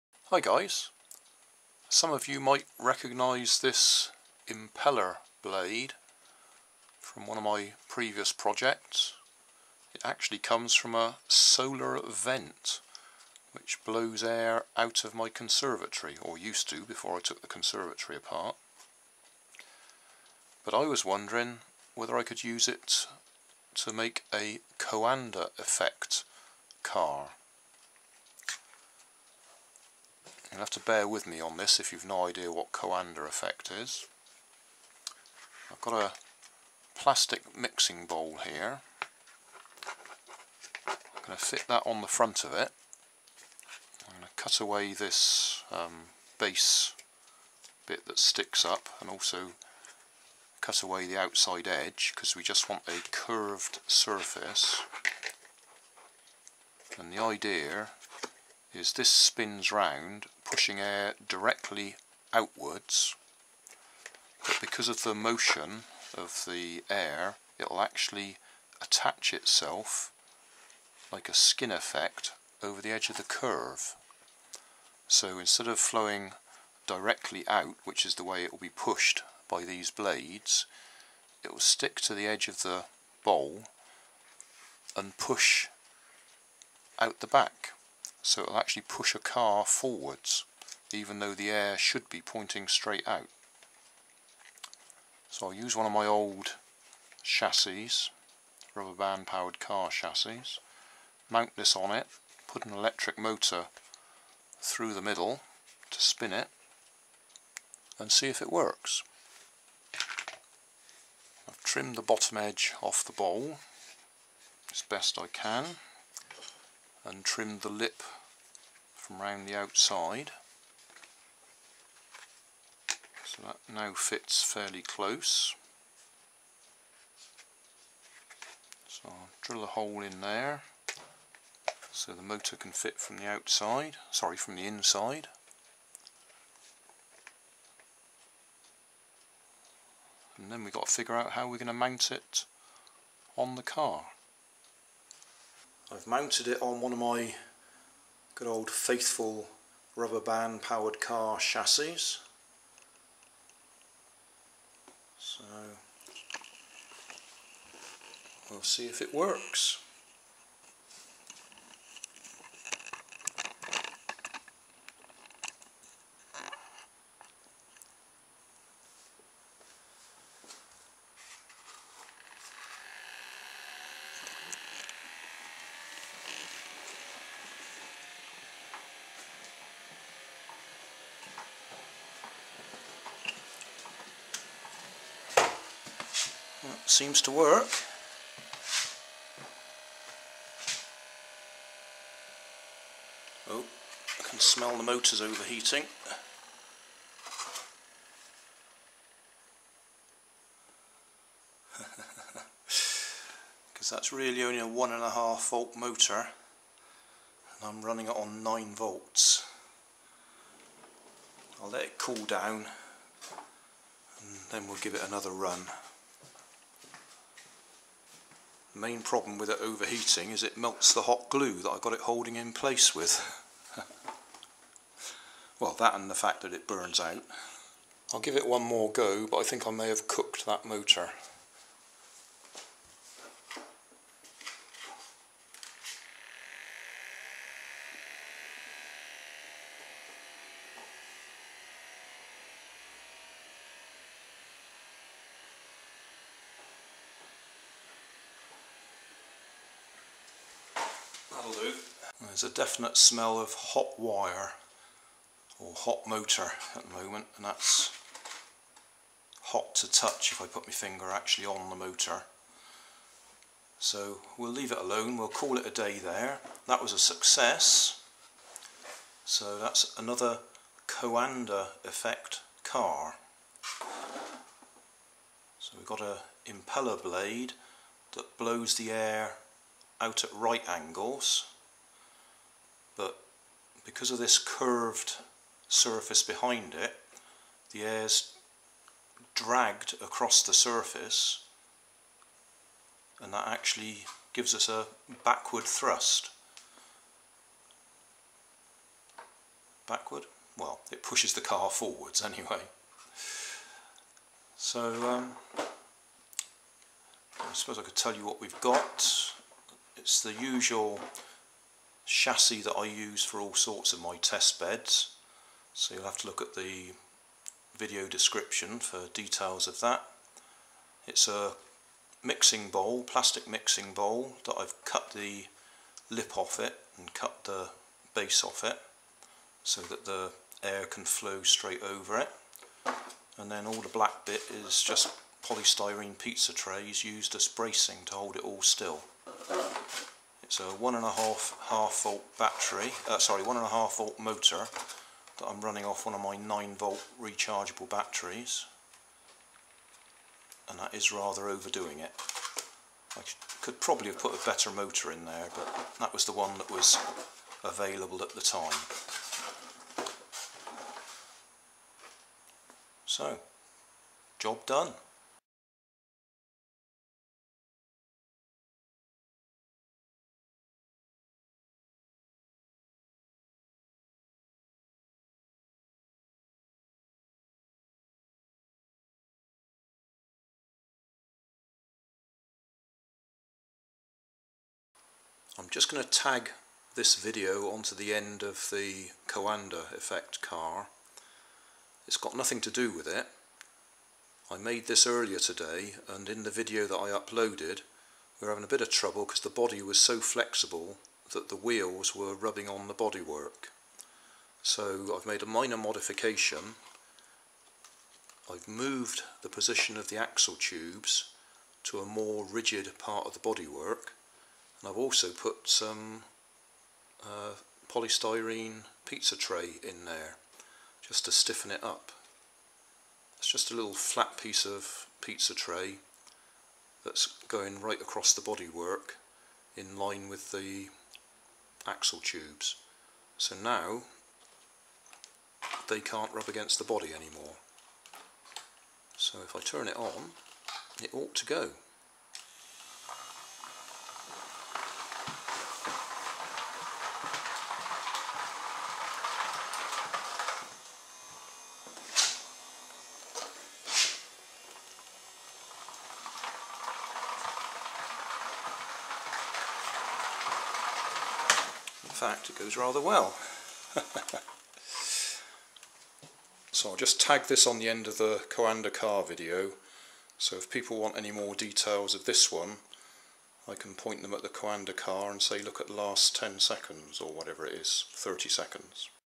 Coanda Effect Car #2
I took this fan blade out of a solar powered air vent and thought it might work for a Coanda effect car. It has taken me several months to find the time to try it. It does work but probably needs a better motor as I managed to overheat the one I used. At the end of this video there is a short clip relating to my Party Plate Fan Car which I have just tagged on here to save uploading another video. The Party Plate Fan Car video is

I think there are two effects in action here. The spinning impeller blade pushes the air out from it's centre so that creates a partial vacuum in front of the car. Air pressure around the car would tend to push the car forwards to fill the gap. At the same time the moving air will tend to 'stick' to the curved surface so it is directed backwards creating a thrust effect. The best example I can give is the old trick of trying to blow a candle out by blowing through a plastic funnel. The candle is only affected if the rim of the funnel is pointed at the flame.

If you want to get very technical with Coanda effect you might like this video although he uses the spoon in water demonstration which many will tell you is incorrect, it looks right and has the right result but is more to do with surface tension.

Main components
Impeller blade taken from a solar air vent
Plastic mixing bowl
Polystyrene pizza trays
Electric motor
Plastic conduit
4 CDs/DVDs
8 plastic bottle tops
2 drinking straws
2 bbq sticks

Main Tools
Hot Glue Gun
Hobby knife
Saw